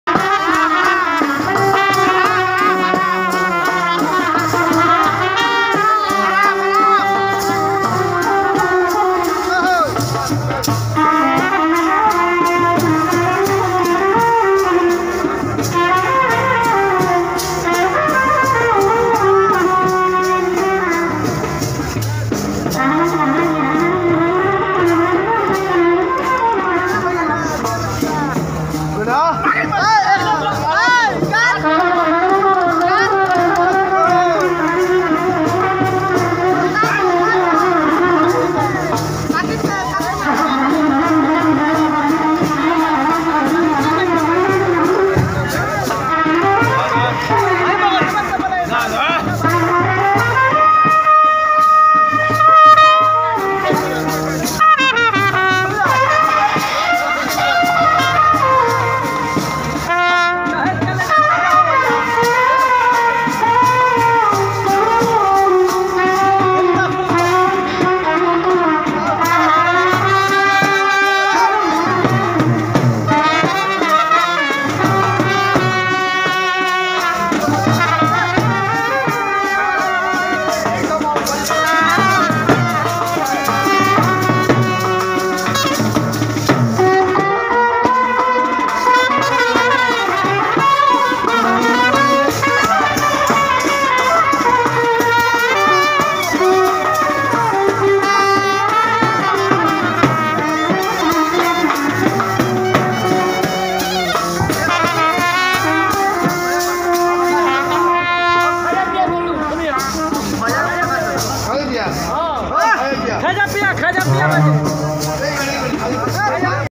Raag Marwa Alaap || Ustad Aslam Saheb ||International Trumpet Player Patna City
Raag Marwa || Trumpet Alaap || International Trumpet Ustad Aslam Master